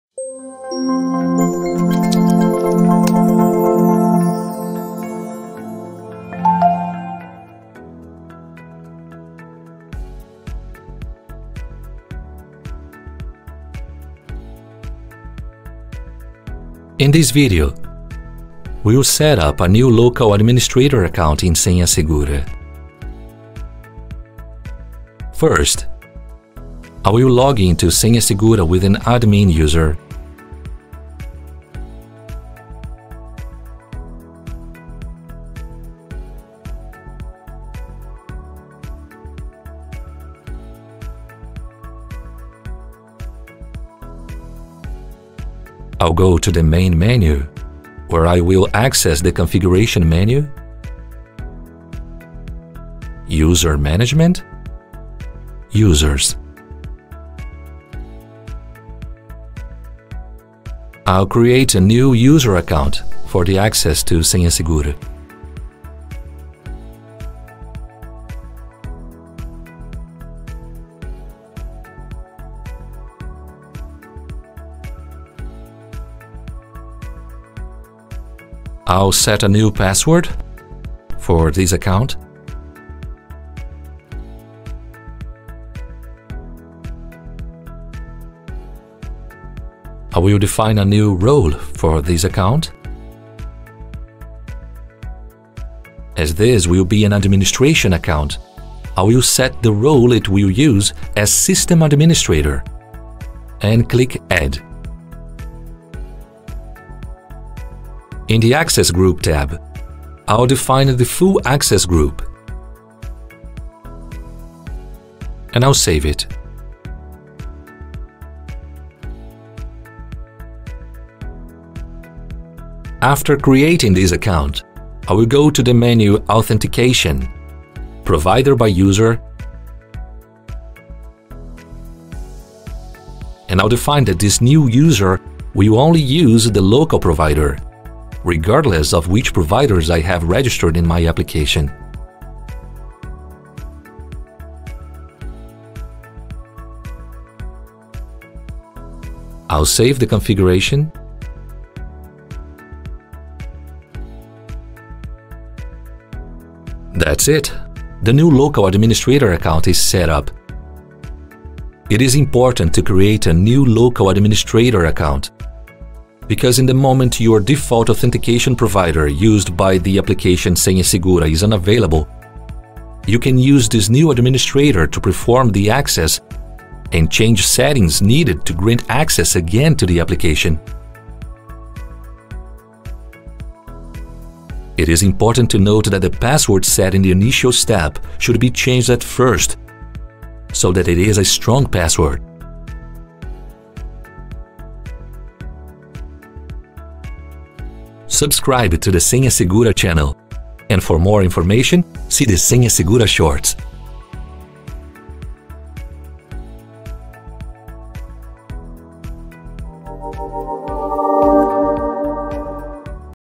How to create new local admin user
In this video, you will check how to create a new local user admin:

1. Log in to senhasegura with admin user in access Settings ➔ User management ➔ Users and click on the New button.
2. Fill in the requested information.
3. In the tab ‘Roles’ select ‘System Administrator’ and click on Add.
4. In the tab ‘Access group’ select ‘Full access’.
5. After creating the account access Settings ➔ Authentication➔Providers by the user and click on the New button.
6. In the new screen, select the user in User*,  in Authentication provider select Local, select 1 in Order*, and if it is enabled or not.
7. Click on ‘Save’ to finish.

It is important to create a new local administrator account because at the moment your default authentication provider used by the application senhasegura is unavailable you can use this new administrator to perform the access and change settings needed to grant
access again to the application.